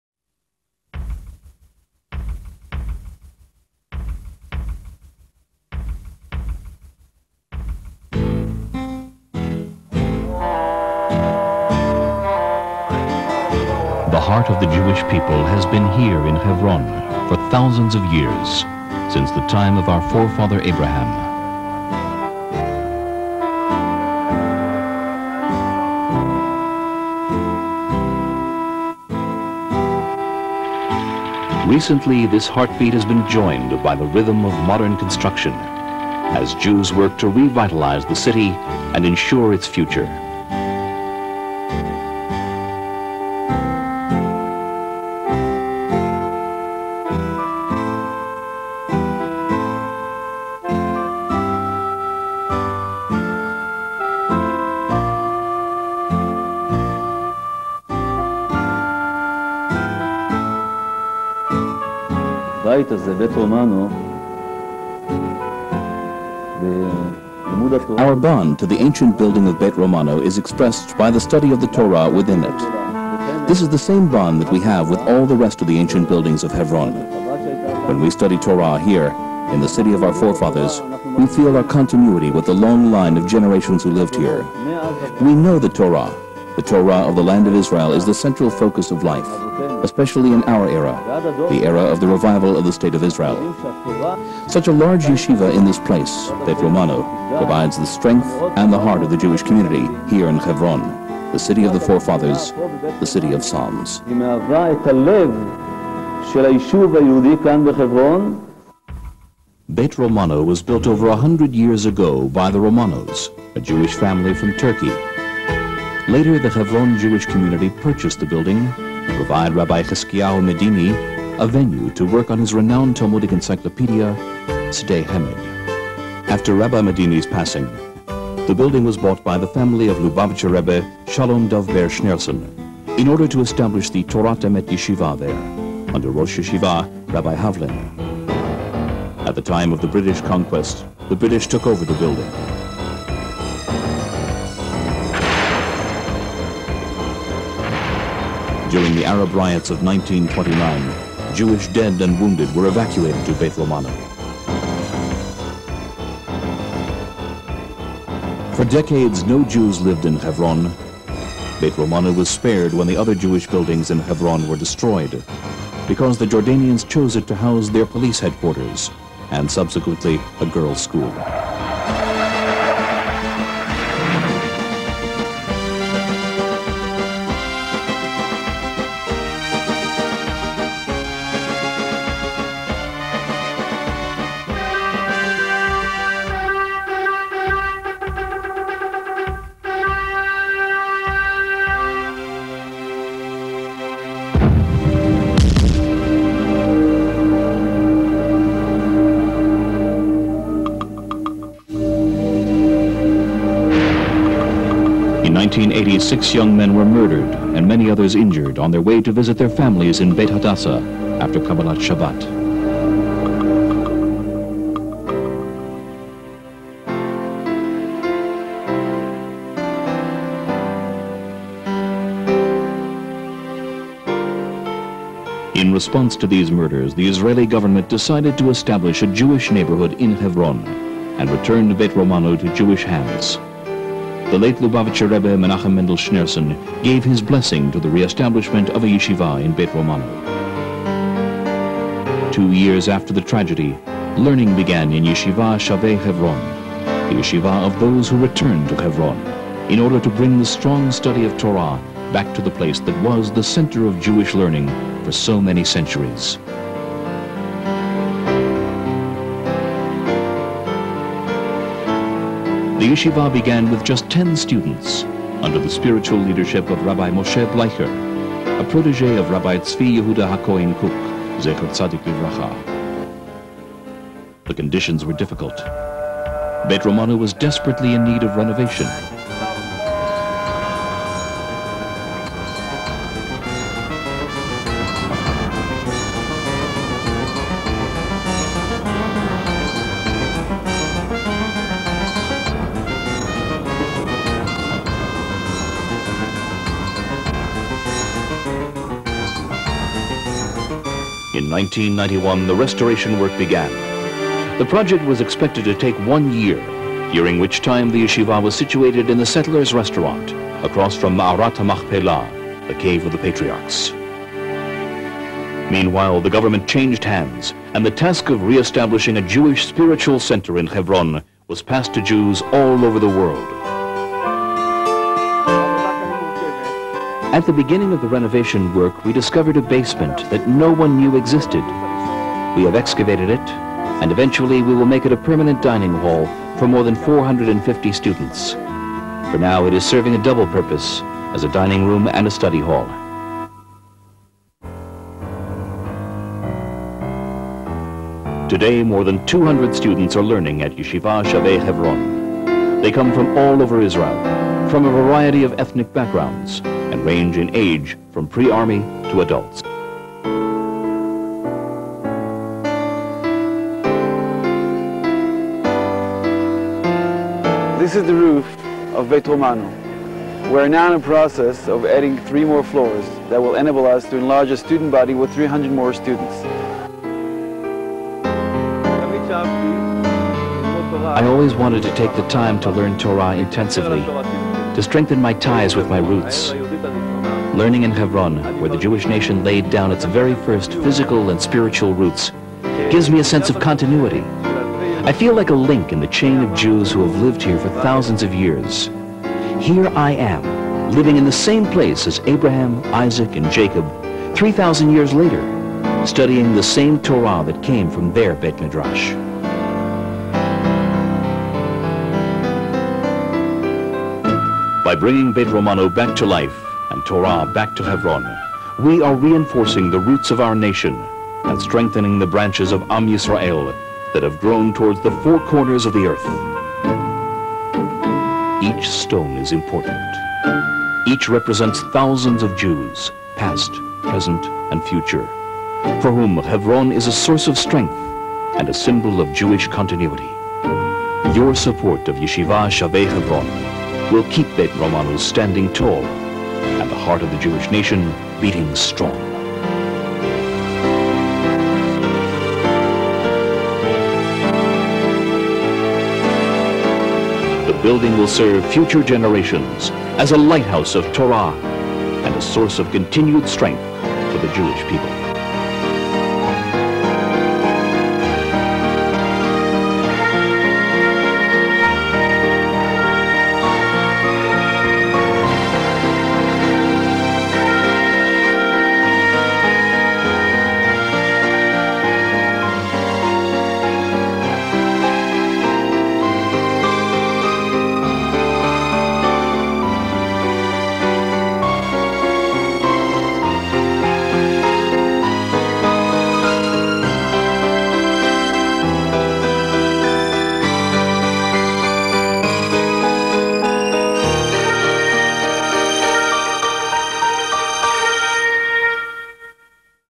Yeshivat Shavei Hevron PR Film circa late 90s
This film was created in approximately 1997 as a PR film for Yeshivat Shavei Hevron. I (Toby Klein Greenwald) was commissioned by Gideon Margalit to help with the script. It was directed and produced by Bruno Bisraor, Images Productions. The composer of the soundtrack, Aaron Gurov, originally from Russia, who lived with his family in Tekoa, was murdered by terrorists on Feb. 25, 2002. He had been a gifted composer. Here is a page in Hebrew that gives more information about him:
I saved the video in its old VHS format and on August 9, 2022, it was transferred to digital format by Daniel Saas